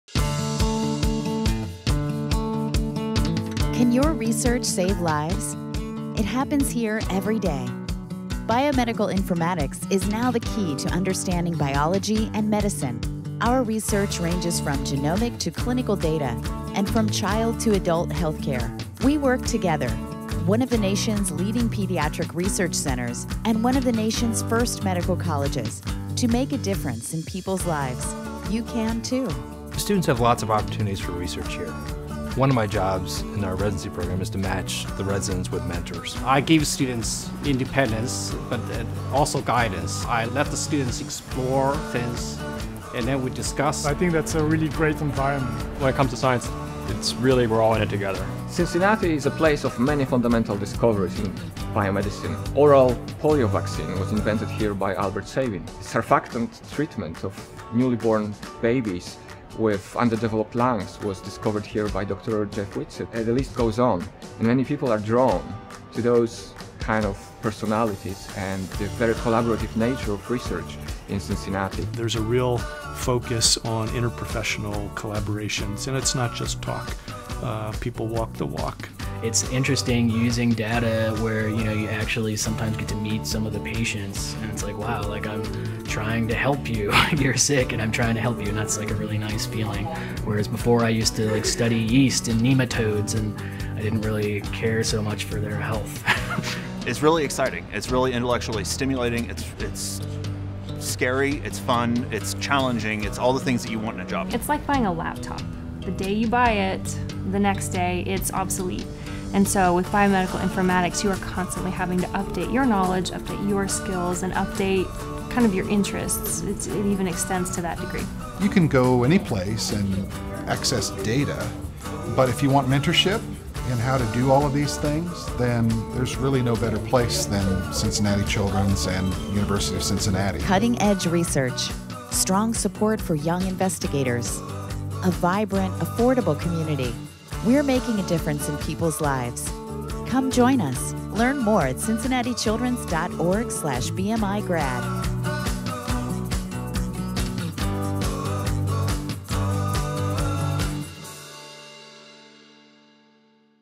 Biomedical Informatics Graduate Certificate and PhD Program | Cincinnati Children's and UC
Cincinnati Children's | University of Cincinnati

Can your research save lives? It happens here every day.

Biomedical informatics is now the key to understanding biology and medicine. Our research ranges from genomic to clinical data and from child to adult healthcare.

We work together: one of the nation's leading pediatric research centers and one of the nation's first medical colleges to make a difference in people's lives.

Daniel Schauer, MD, internist and clinical informatics researcher: "Students have lots of opportunities for research here. One of my jobs in our residency program is to match the residents with mentors."

Jun Ma, PhD, developmental biologist: "I give students independence but also guidance. I let the students explore, and then we discuss."

Guillaume Labilloy, student: "I think it's a really great environment."

David Cheung, research technician: "It's really, we're all in it together."

Jarek Meller, PhD, graduate program director: "Cincinnati is a place of many fundamental discoveries in biomedicine. Oral polio vaccine was invented here by Albert Sabin. Surfactant treatment of newborn babies with underdeveloped lungs was discovered here by Dr. Jeff Whitsett. And the list goes on. Many people are drawn to those kind of personalities and the very collaborative nature of research in Cincinnati."

Mark Eckman, MD, internist and clinical informatics researcher: "There's a real on interprofessional collaborations, and it's not just talk. People walk the walk."

Matthew Weirauch, PhD, computational biologist with emphasis on genomics: "It's interesting using data where, you know, you actually sometimes get to meet some of the patients. And it's like, 'Wow, I'm trying to help you. You're sick and I'm trying to help you.' And that's a really nice feeling, where before I used to study yeast in nematodes. I didn't really care so much for their health."

Phillip Dexheimer, student: "It's really exciting. It's really intellectually stimulating. It's scary. It's fun. It's challenging. It's all the things that you want in a job."

Rebekah Karns, PhD, research associate: "It's like buying a laptop. The day you buy it, the next day, it's obsolete. And so, with biomedical informatics, you are constantly having to update your knowledge, update your skills and update kind of your interests. It even extends to that degree."

Andy Spooner, MD, chief medical information officer: "You can go anyplace and access data.But if you want mentorship in how to do all of these things, and there's really no better place than Cincinnati Children's and the University of Cincinnati."

Cutting-edge research. Strong support for young investigators. A vibrant, affordable community. We're making a difference in people's lives. Come join us.